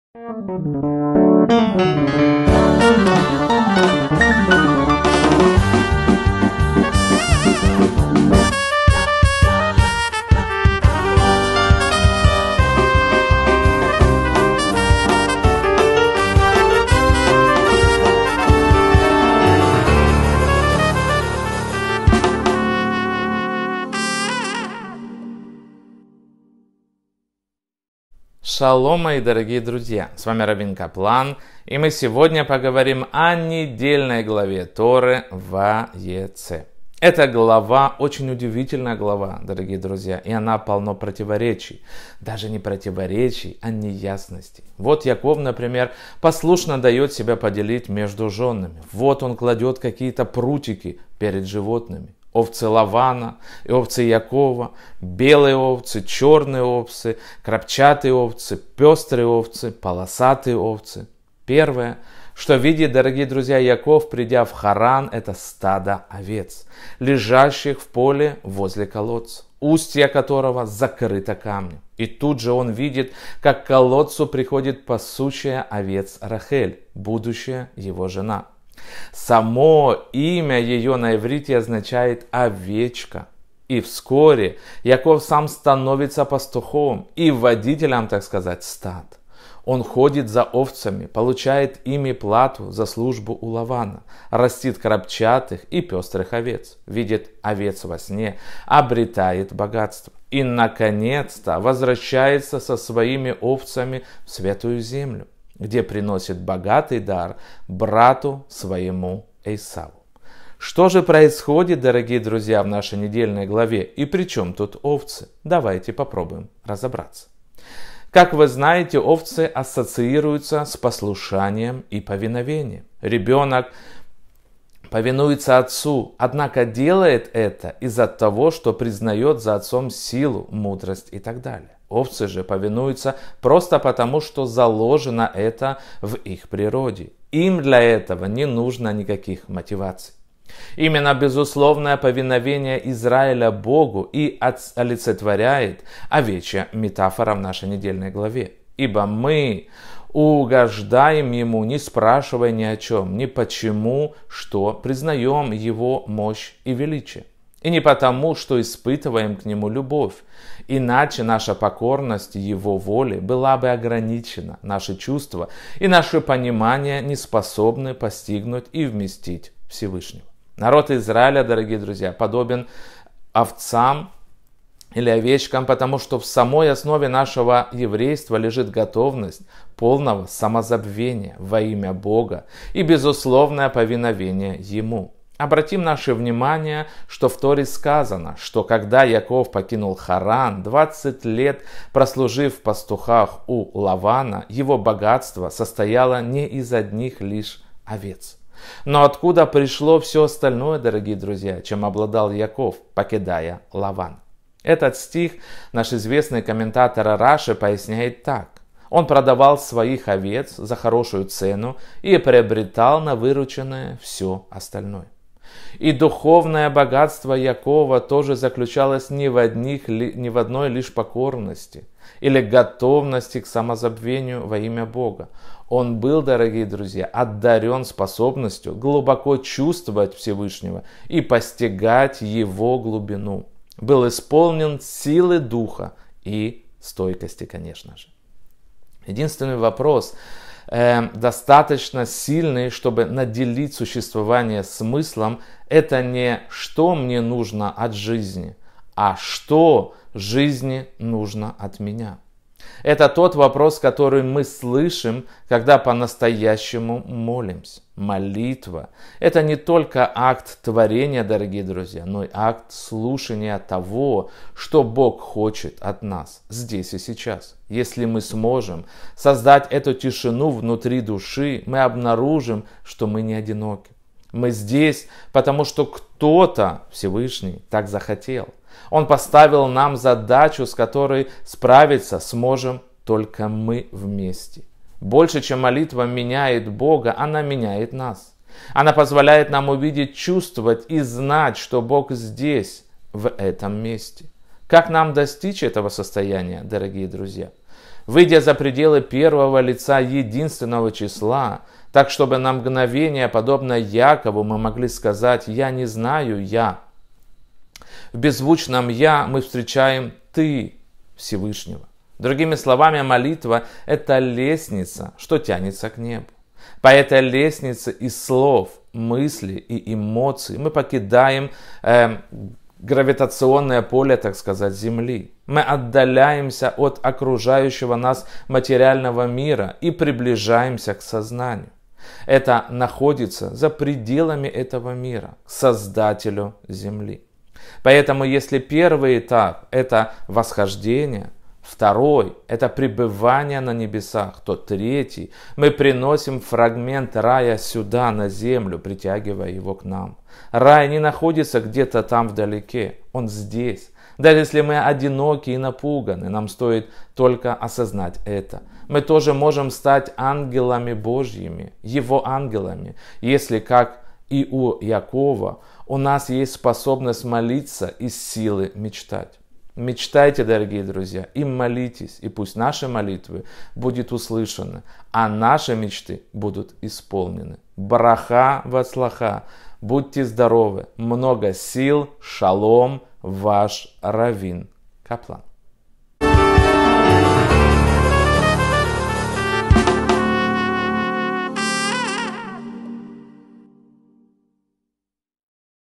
🙏 Молитва - это лестница, что тянется к небу. Ваеце 5782 | Рав Бенцион Каплан
Шалом, дорогие друзья! С ввми раввин Каплан, и мы сегодня поговорим о недельной главе Торы Ваеце.

Овца, безусловно, по праву считается одним из самых «еврейских» животных. Впервые мы встречаемся с ней в Торе, ведь скотоводство, наряду с земледелием — одно из основных занятий патриархов, а обладание большими стадами мелкого рогатого скота — важнейший показатель богатства.

Многие ключевые сюжеты первой из книг Торы, Берешит связаны с овцами, которые называются на иврите словом «цон», т. е. «мелкий рогатый скот».